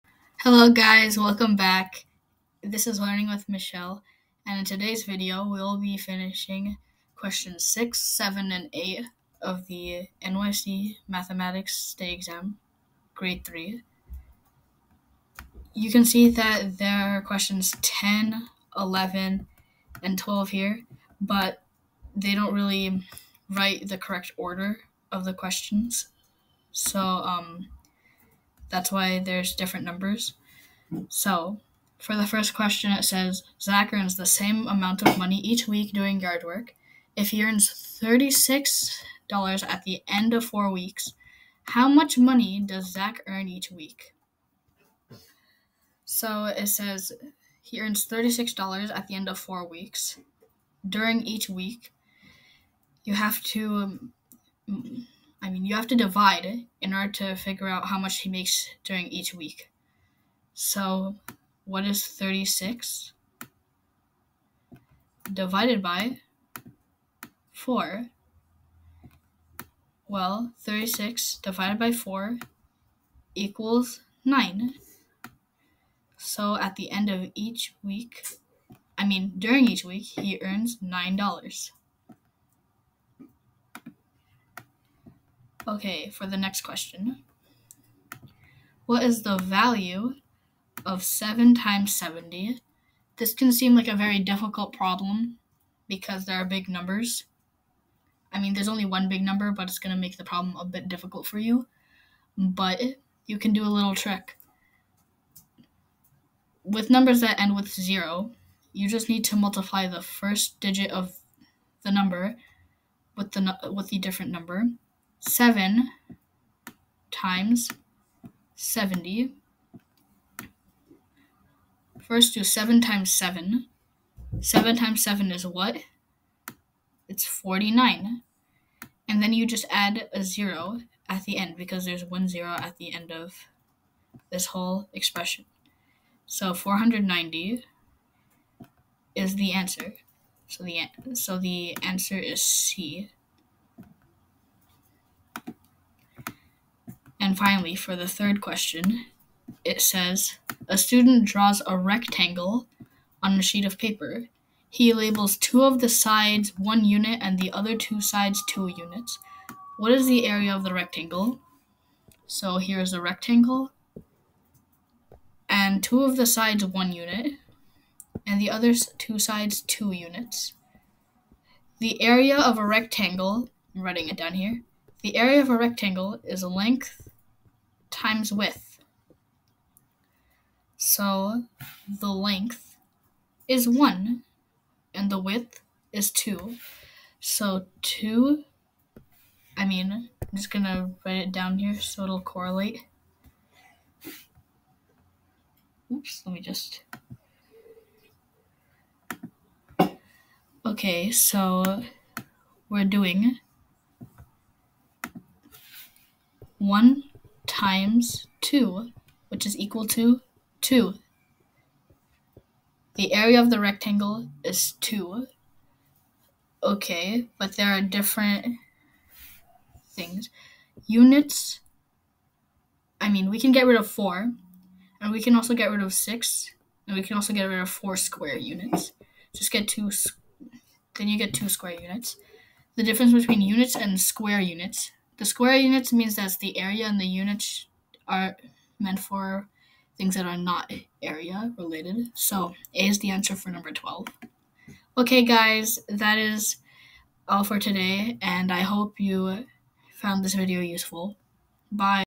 3rd Grade Math State Test 2022 Questions 10 - 13
3rd Grade Math State Test 2022 Questions 10 - 13
This video shows the questions and answers for questions 10 - 13 of the 3rd Grade New York State Math Exam. To access the state exam :
- click on '2022,'
- go on 'Grade 3,"
- select 'Released Test Questions.'